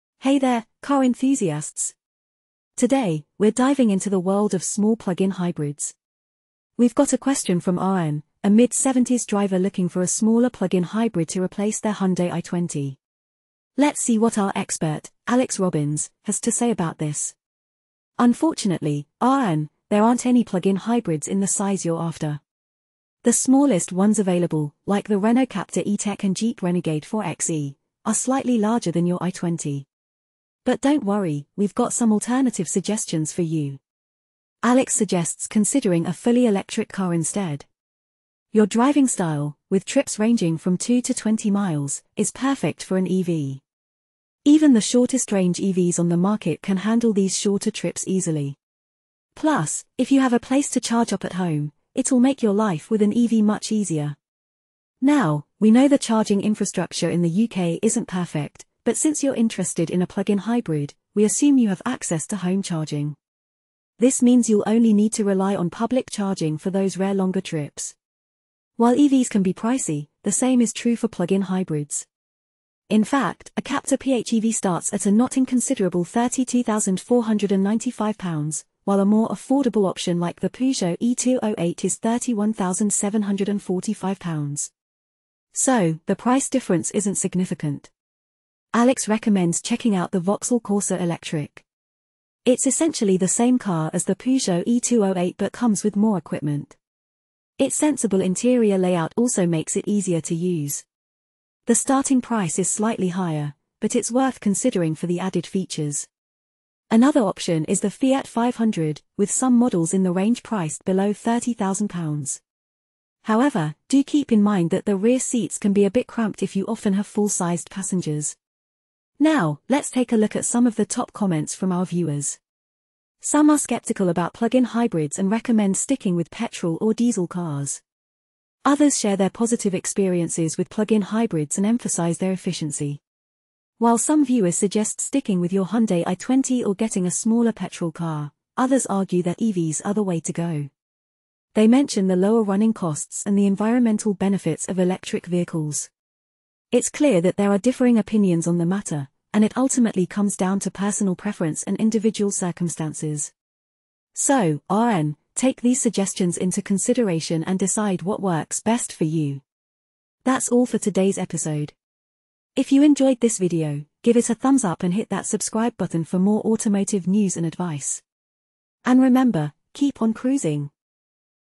Ask the Expert: Which small plug-in hybrid should I buy?
The expert in this article responds to a reader's question about which small plug-in hybrid car to buy. The reader, who is in their mid-70s, drives between two and 20 miles and is looking for a smaller car than their current Hyundai i20. The expert explains that there are no plug-in hybrid cars available in the size the reader is looking for. The smallest plug-in hybrids on the market are the Renault Captur E-Tech and Jeep Renegade 4xe, which are taller and slightly larger than the i20. The expert suggests considering a fully electric car instead, as the reader's driving style would be well-suited to an EV. They mention the Peugeot e-208 and Vauxhall Corsa Electric as potential options, with the latter having a more sensible interior layout. The expert acknowledges that EVs can be costly to buy, but points out that PHEVs are similarly expensive. They conclude by suggesting that the reader either stick with their current Hyundai or consider a smaller petrol car if they decide to change. The comments section includes a range of opinions, with some supporting the expert's advice and others advocating for petrol or diesel cars instead.